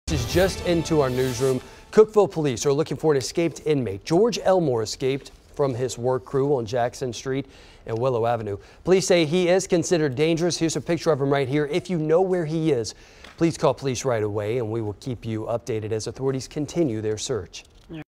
Escaped inmate reported in Cookeville
Authorities in Putnam County reported an inmate escaped from a jail work crew early Wednesday morning.